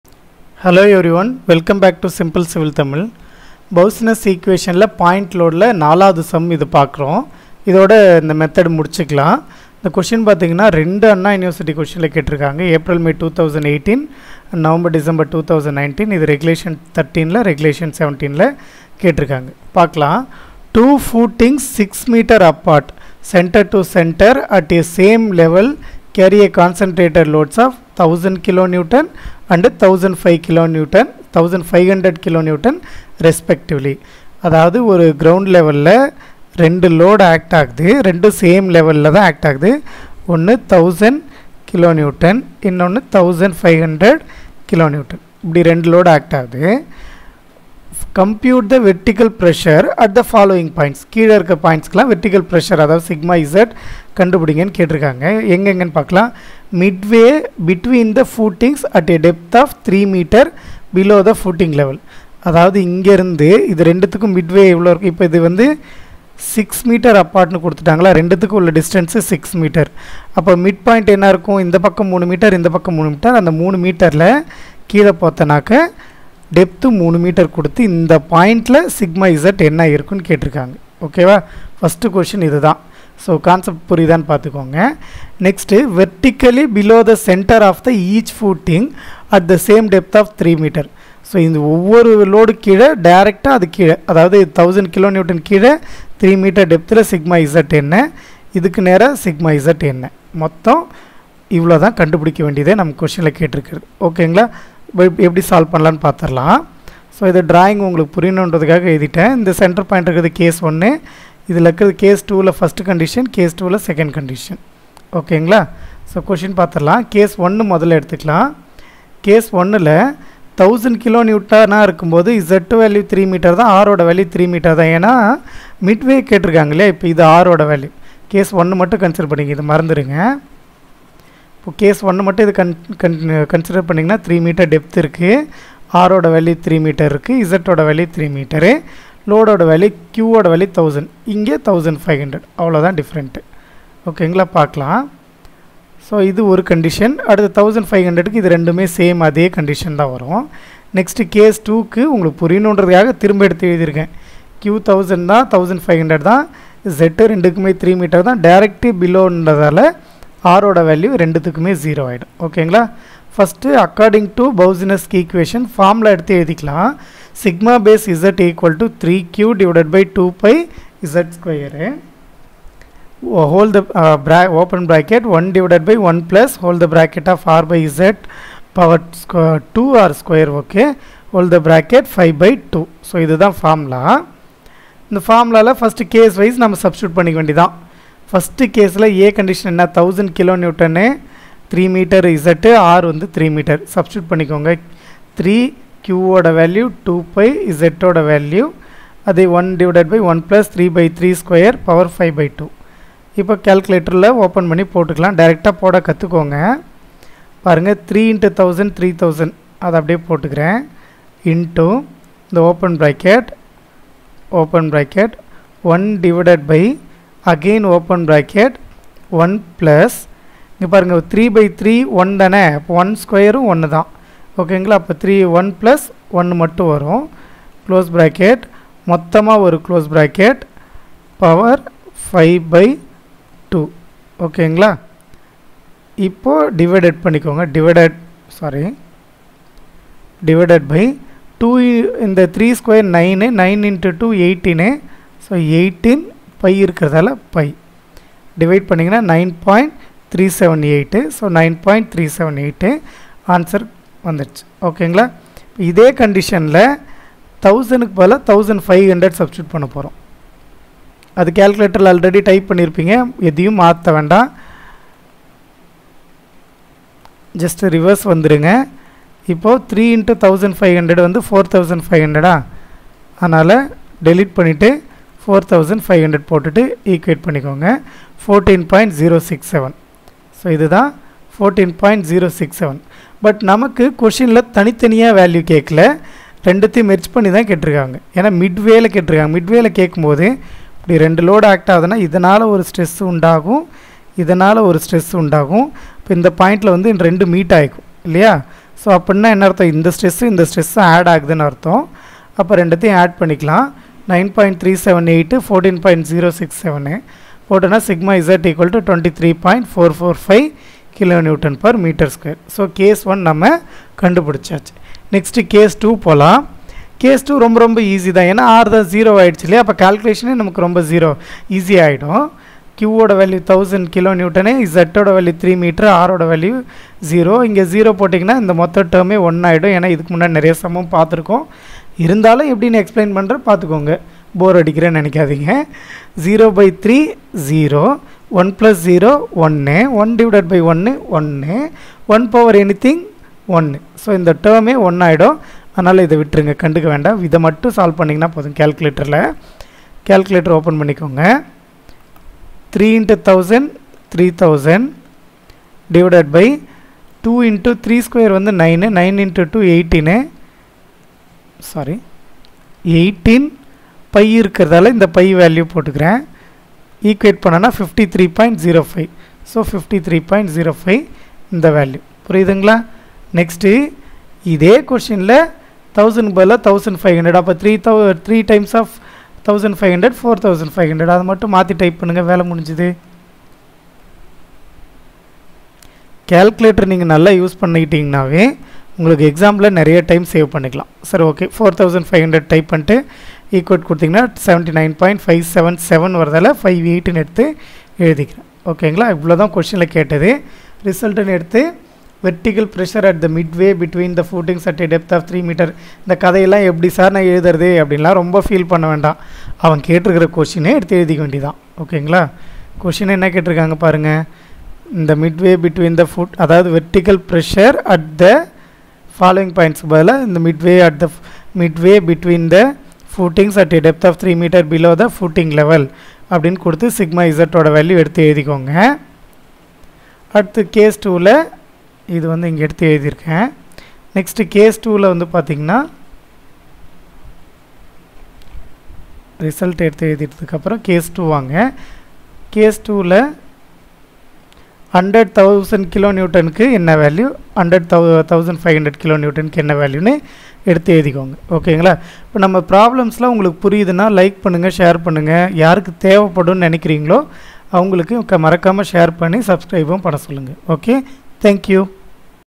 Boussinesq's equation point load problem 4
Two footings 6 m apart (c/c distance) at same level carry
concentrated loads of 1000 kN and 1500 kN respectively.
Compute the vertical pressure at the following points.
i.
ii.
Midway between the footings at a depth of 3 m below
the footing level.
Vertically below the centre of each footing at the
same depth of 3 m.